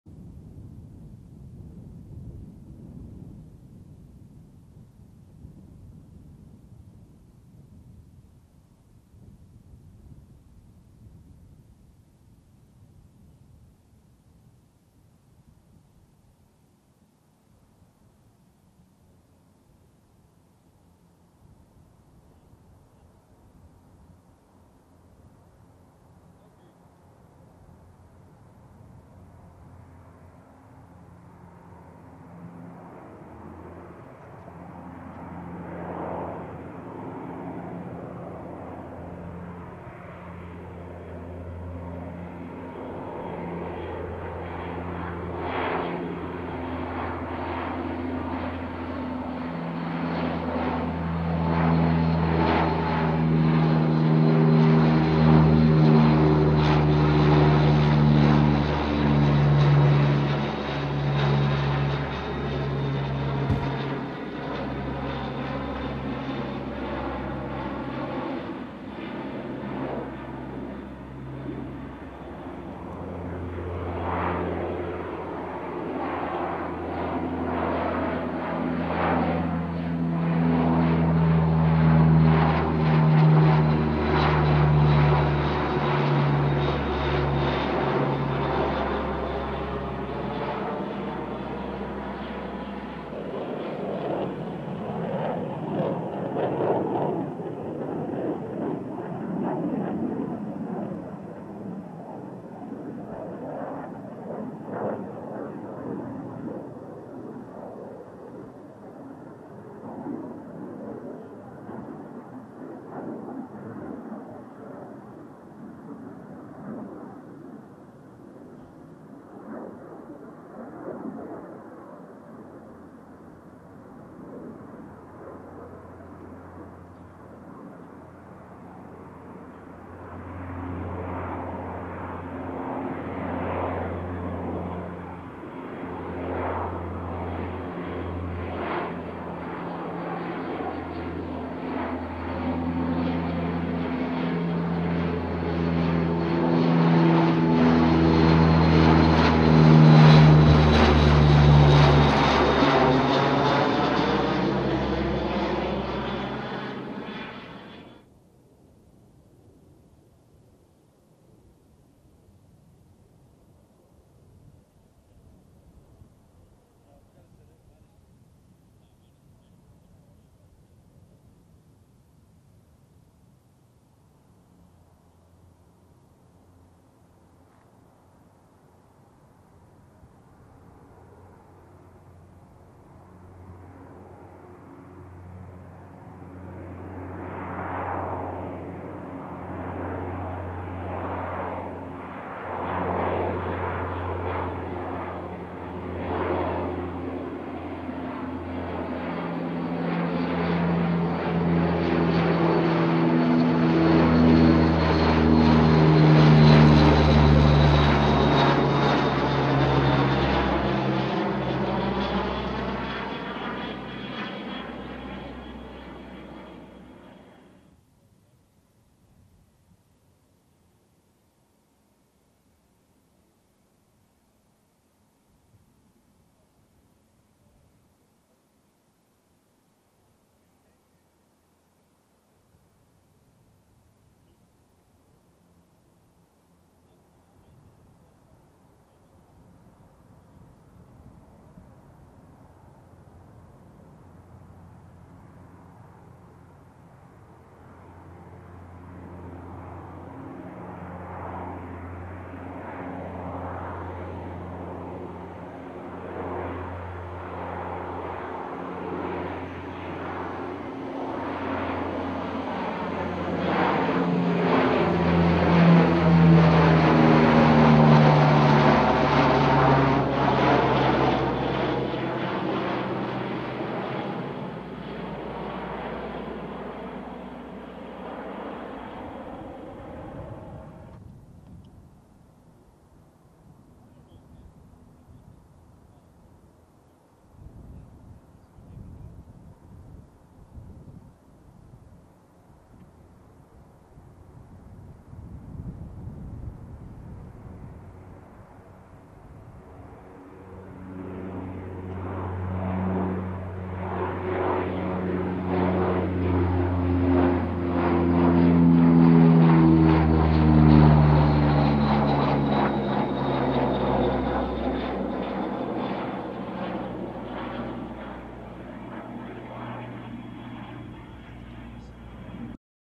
US Navy C-2 Greyhound - NAF El Centro
A C-2 Greyhound practices pattern work and touch-and-go landings at NAF El Centro.